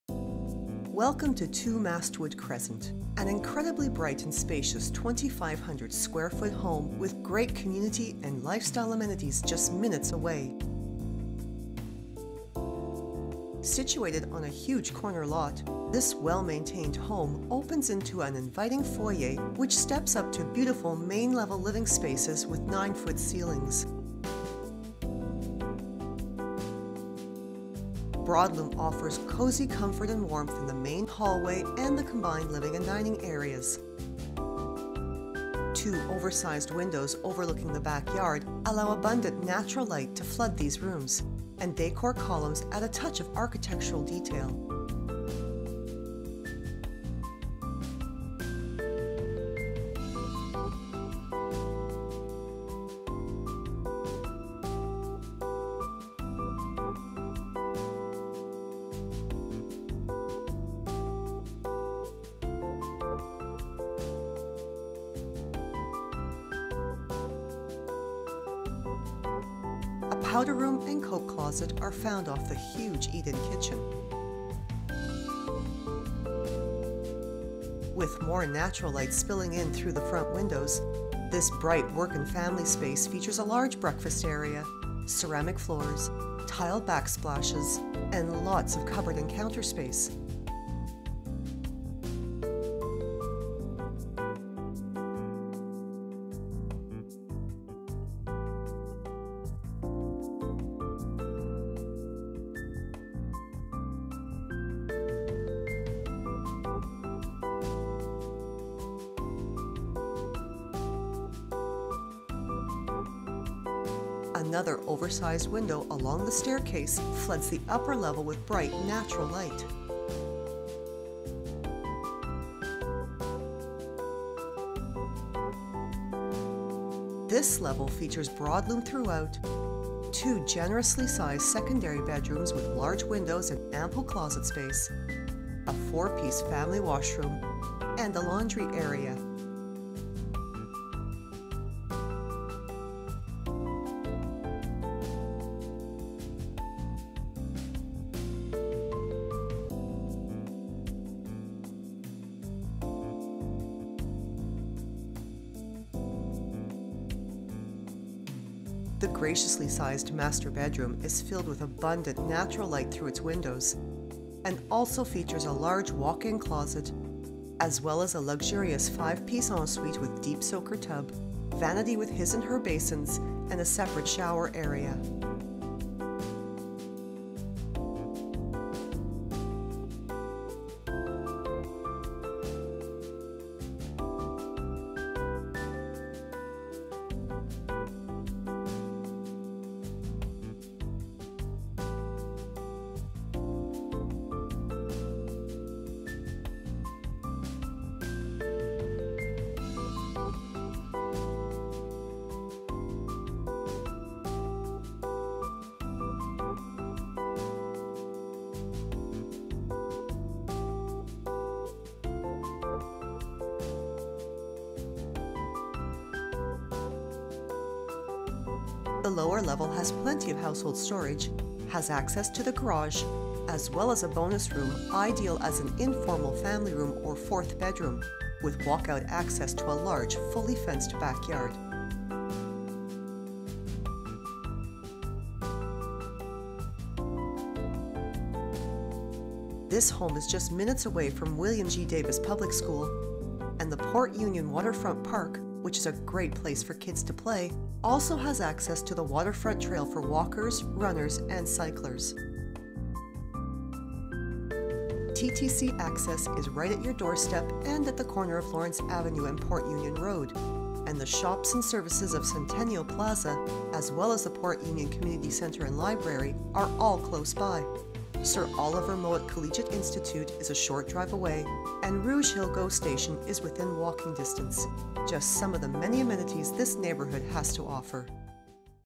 2 Mastwood Cres, Toronto, Open House Video Tour
Welcome to 2 Mastwood Crescent, an incredibly bright and spacious 2,500 sq. ft. home with great community and lifestyle amenities just minutes away.

Situated on a huge corner lot, this well-maintained home opens into an inviting foyer which steps up to beautiful main level living spaces with 9 ft. ceilings.

Broadloom offers cozy comfort and warmth in the main hallway and the combined living and dining areas. Two oversized windows overlooking the backyard allow abundant natural light to flood these rooms, and decor columns add a touch of architectural detail.

A powder room and coat closet are found off the huge eat-in kitchen. With more natural light spilling in through the front windows, this bright work and family space features a large breakfast area, ceramic floors, tiled back splashes, and lots of cupboard and counter space.

Another oversized window along the staircase floods the upper level with bright, natural light. This level features broadloom throughout; two generously sized secondary bedrooms with large windows and ample closet space; a 4-pc family washroom; and a laundry area.

The graciously sized master bedroom is filled with abundant natural light through its windows, and also features a large walk-in closet, as well as a luxurious 5-pc ensuite with deep soaker tub, vanity with his and her basins, and a separate shower area.

The lower level has plenty of household storage, has access to the garage, as well as a bonus room ideal as an informal family room or fourth bedroom, with walk-out access to a large fully fenced back yard.

This home is just minutes away from William G. Davis Public School - and the Port Union Waterfront Park, which is a great place for kids to play, also has access to the Waterfront Trail for walkers, runners, and cyclers.

TTC access is right at your doorstep and at the corner of Lawrence Avenue and Port Union Road, and the shops and services of Centennial Plaza, as well as the Port Union Community Centre and Library, are all close by. Sir Oliver Mowat Collegiate Institute is a short drive away, and Rouge Hill GO Station is within walking distance, just some of the many amenities this neighbourhood has to offer.